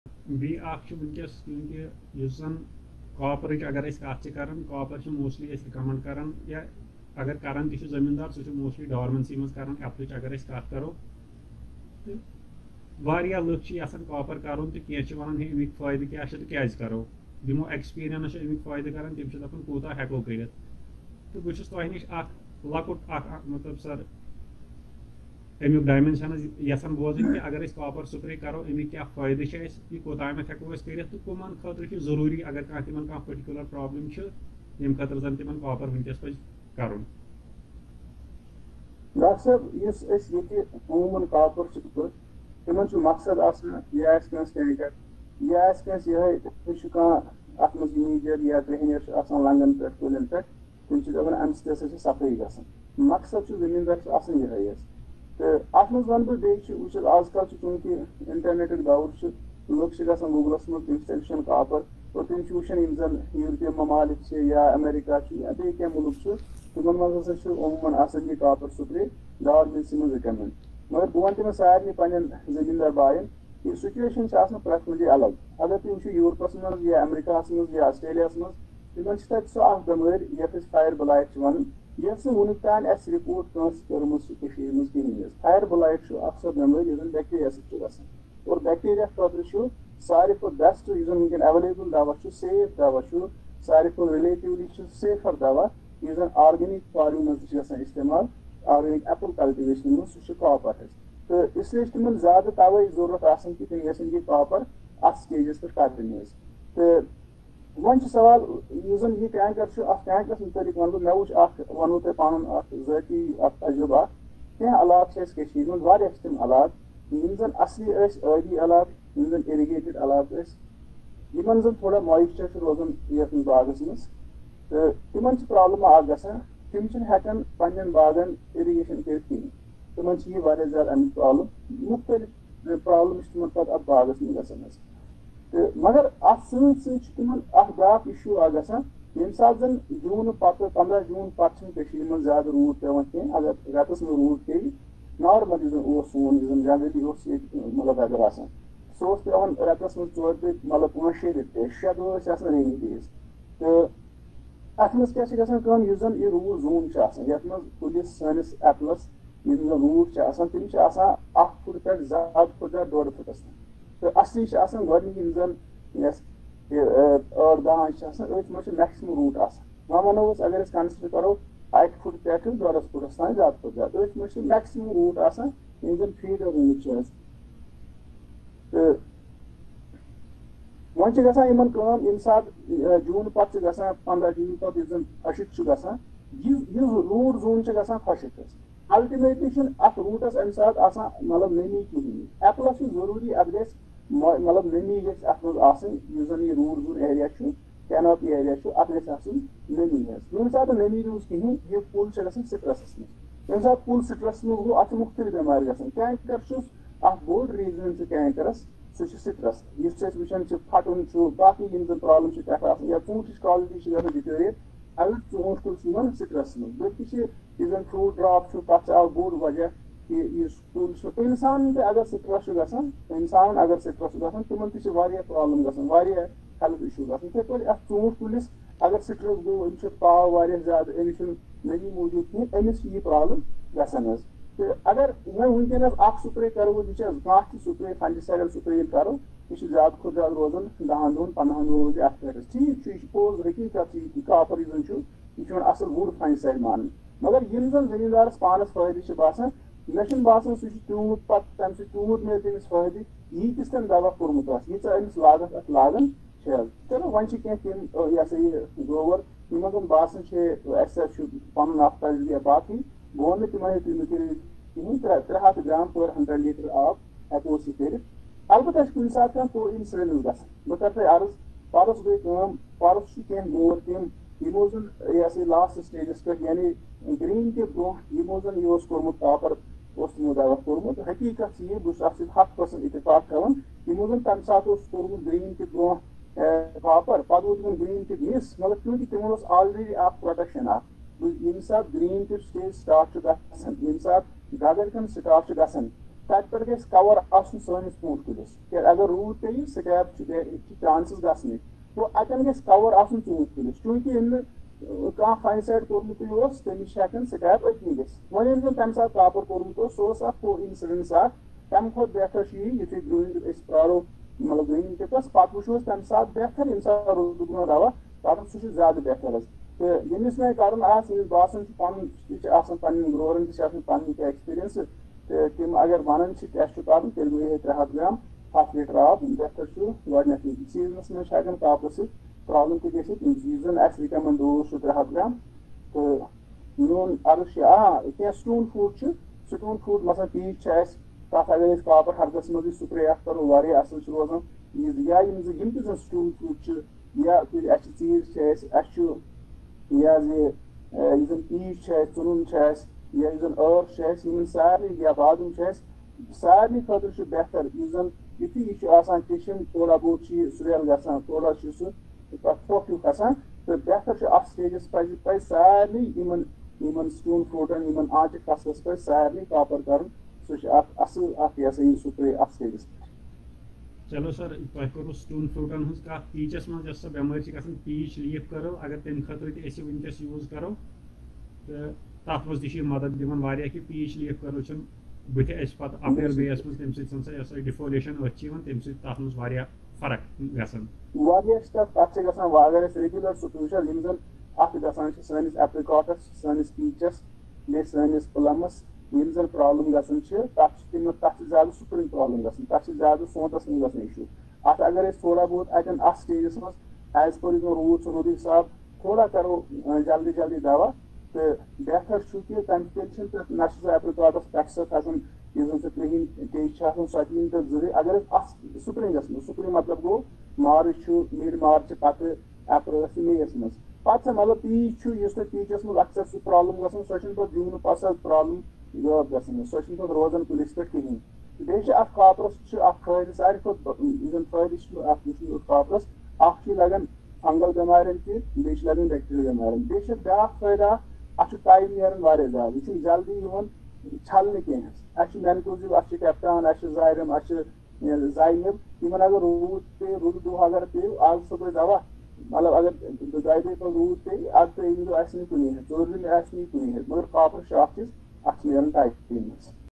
Copper Spray in Fruit Trees by Dr. Tariq Rasool | Benefits of Copper Spray | Canker management
The video is a part of live conversation with Dr. Tariq Rasool on current issues like fertilizer, copper, oil spray and scab Management

Important Framing Tools
Grafting Tape
Pruning Saw
Soil pH Meter
Grafting Tools